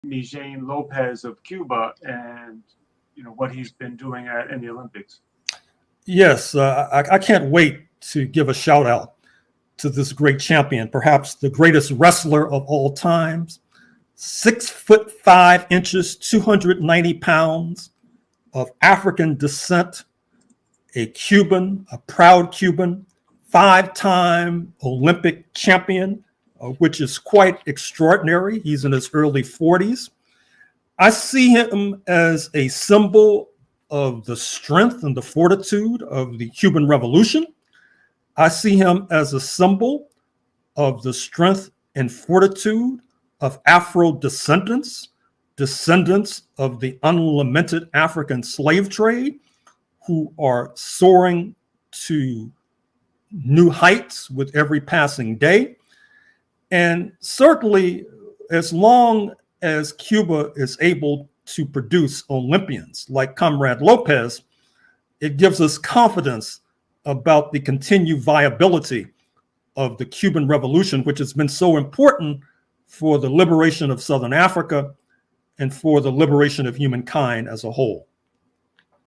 Gerald Horne - Historic symbolism & importance of the Cuban Olympic Wrestling Champion Mijaín López
August 7, 2024, Gerald Horne discussing the historic symbolism and importance of the Cuban Olympic Wrestling Champion Mijaín López.